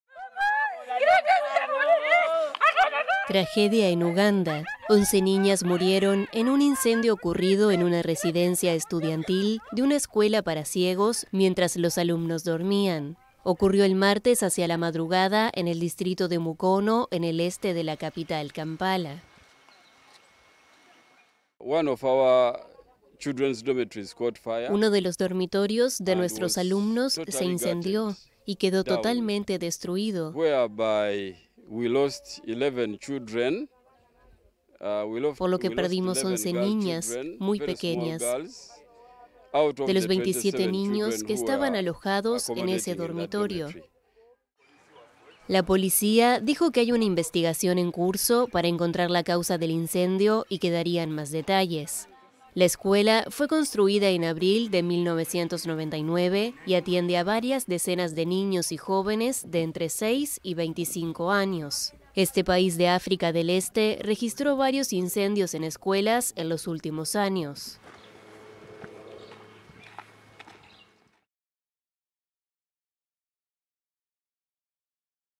Once escolares muertos en el incendio de una escuela de Uganda | AFP
Once niñas murieron en un incendio ocurrido en una residencia estudiantil de una escuela para ciegos de Uganda, el martes de madrugada, mientras los alumnos dormían.